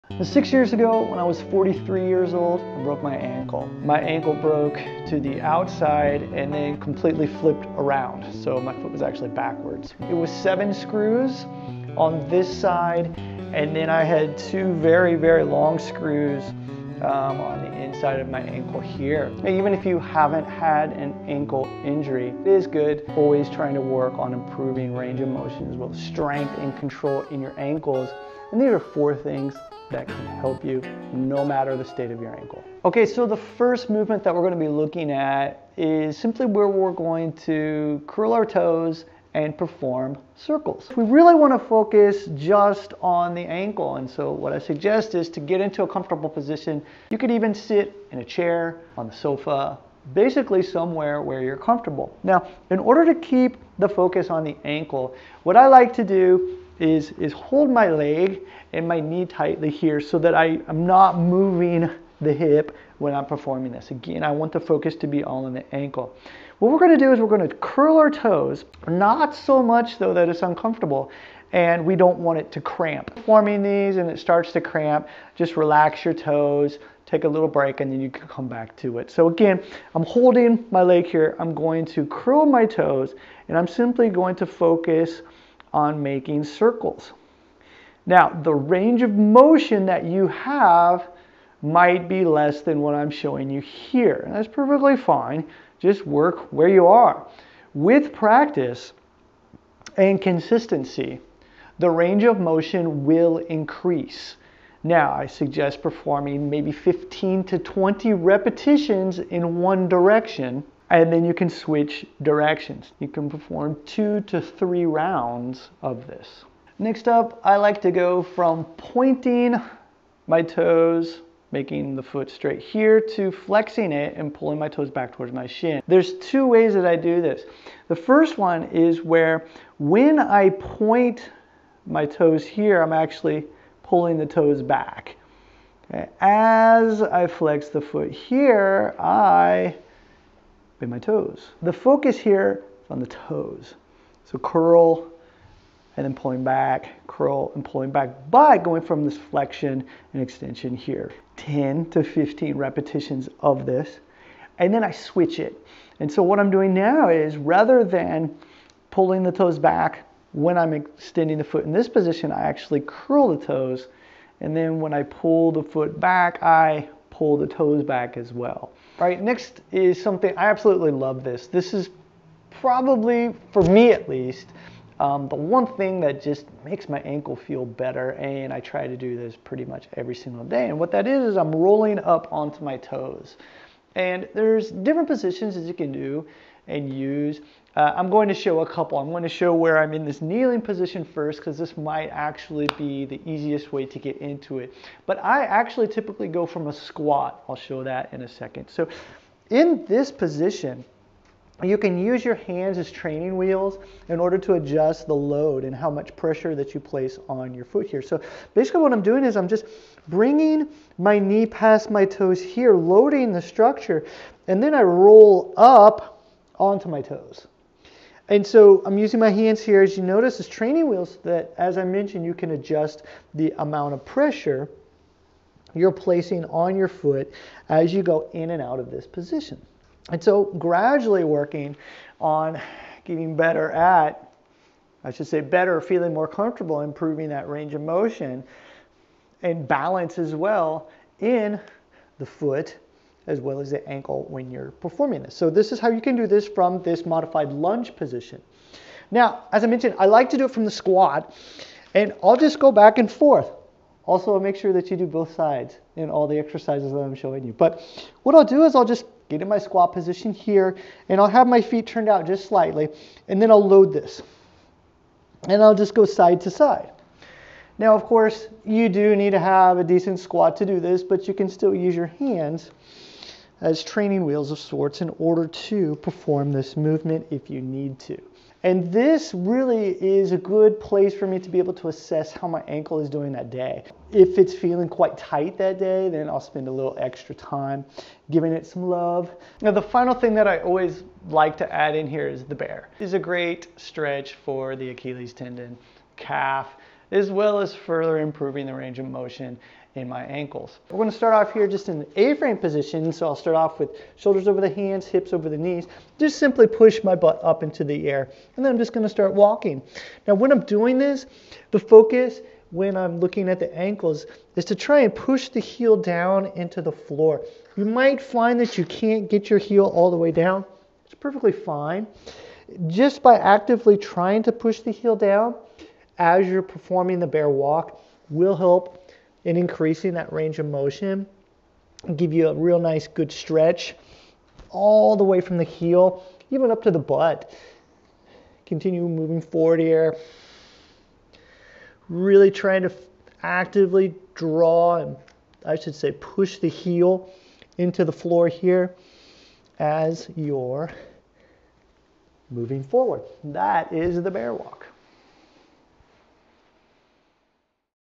4 Ankle Mobility Exercises to Unlock Your Tight Ankles
If your ankles are stiff from an injury like Ryan, or your ankles are tight from sitting for long periods throughout the day, the exercises Ryan shares in this video will help you loosen them while improving your ankle strength and balance.

We really hope you found this video useful. We'll do our best to help.